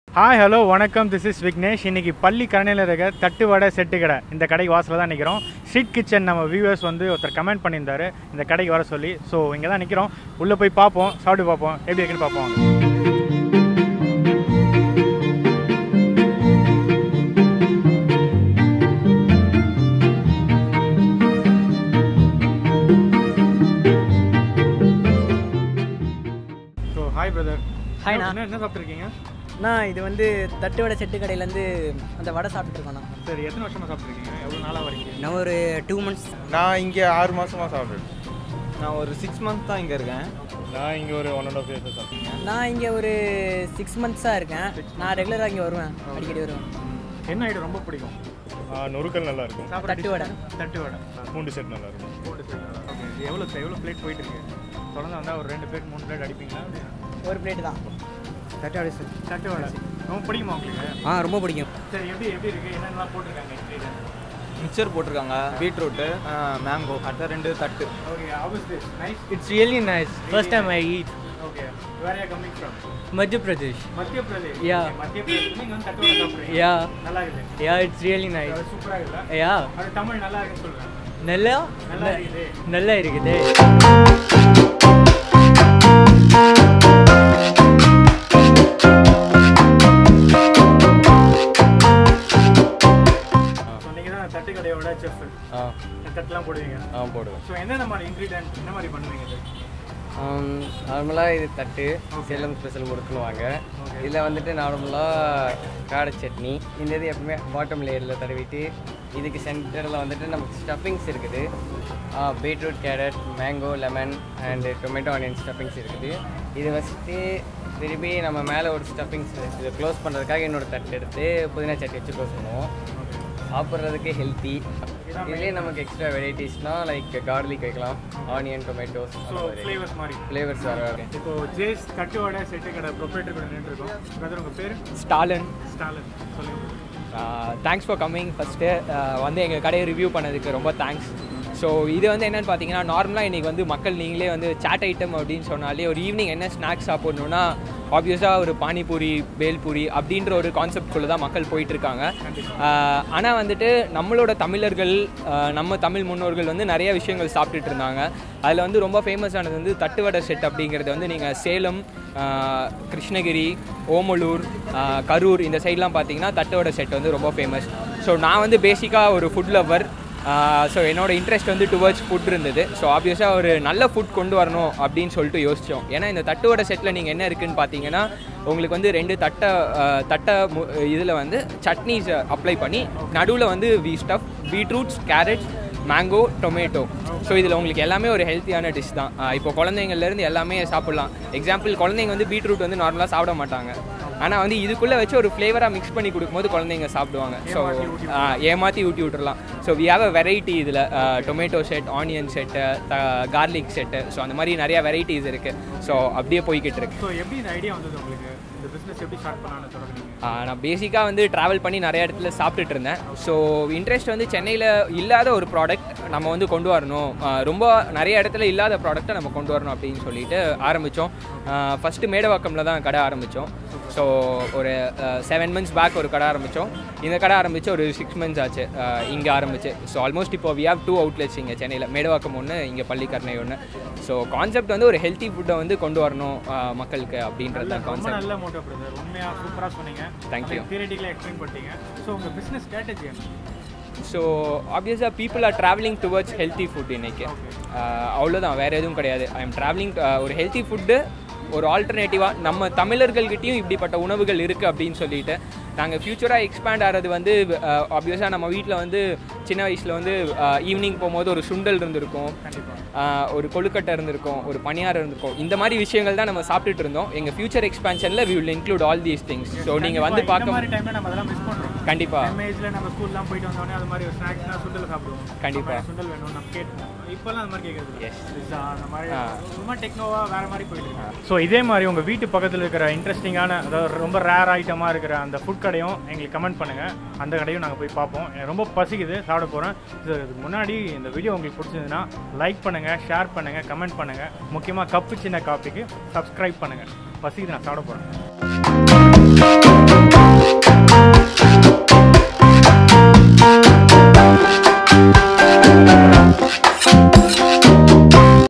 Settu Kadai | Tamil Food Review Show | Street Kitchen | 4K
An Exclusive Show About StreetFood In Budget - SettuKadai Special

Anchor: Vicky
Producer: Vivek
Cameraman: Muthu Selvam
Editor: Faiz
Language: தமிழ்